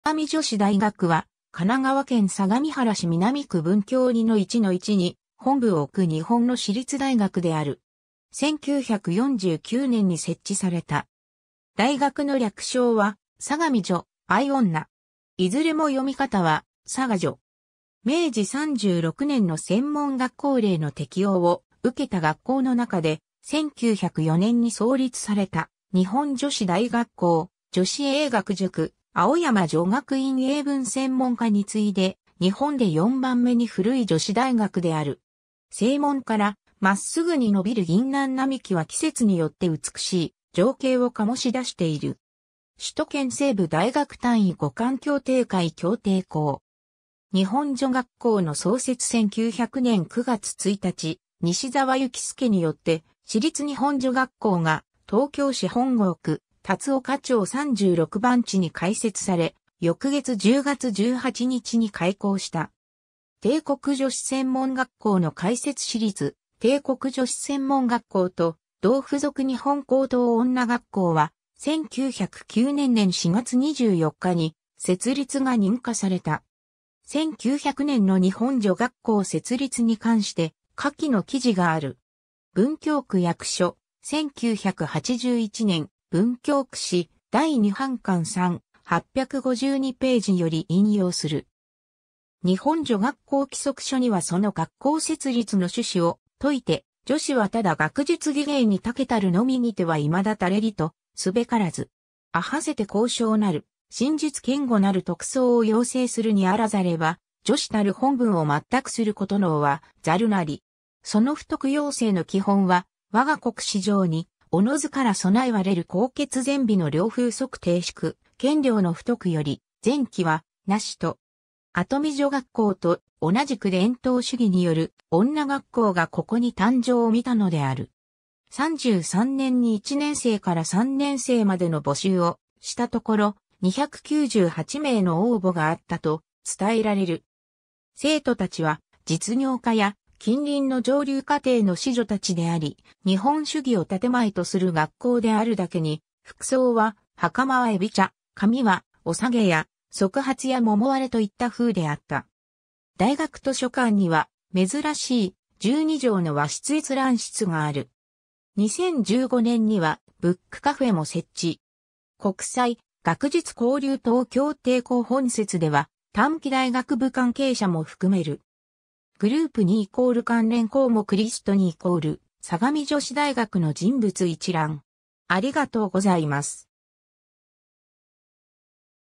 相模女子大学
相模女子大学（さがみじょしだいがく、英語: Sagami Women's University）は、神奈川県相模原市南区文京2-1-1に本部を置く日本の私立大学である。
1949年に設置された。
大学の略称は相模女、相女。
いずれも読み方は「さがじょ」。
 明治36年（1903年）の専門学校令の適用を受けた学校の中で、1904年に創立された日本女子大学校（日本女子大学）、女子英学塾（津田塾大学）、青山女学院英文専門科（青山学院）についで日本で4番目に古い女子大学である。
正門からまっすぐにのびる銀杏並木は季節によって美しい情景を醸し出している。
首都圏西部大学単位互換協定会協定校。
 日本女学校の創設 1900年（明治33年）9月1日、西澤之助によって私立日本女学校が東京市本郷区龍岡町36番地（現在の東京都文京区湯島4丁目1番11号）に開設され、翌月10月18日に開校した。
 帝国女子専門学校の開設 私立帝国女子専門学校と同附属日本高等女学校は、1909年（明治42年）年4月24日に設立が認可された。
 1900年の日本女学校設立に関して、下記の記事がある。
文京区役所、1981年、『文京区史：第2版巻3』、852ページより引用する。
 「日本女学校規則書にはその学校設立の趣旨を説いて、「女子はただ学術技芸に長けたるのみにては未だ足れりとすべからず。あはせて高尚なる心術堅固なる徳操を養成するにあらざれば、女子たる本分を全くすること能はざるなり。その婦徳養成の基本は、我が国史上におのづから備はれる高潔善美の良風即貞淑賢良の婦徳より善きはなし」と。
跡見女学校と同じく伝統主義に拠る女学校がここに誕生をみたのである。
33年に1年生から3年生までの募集をしたところ、298名の応募があったと伝えられる。
生徒達は実業家や近隣の上流家庭の子女達であり、日本主義をたてまえとする学校であるだけに、服装は、袴は海老茶、髪はおさげや束髪や桃割れといったふうであった。
」 　大学図書館には珍しい、12畳の和室閲覧室がある。
2015年にはブックカフェも設置。
 国際・学術交流等協定校 本節では短期大学部関係者も含める。
 | group2 = 関連項目 | list2 = 相模女子大学の人物一覧 }}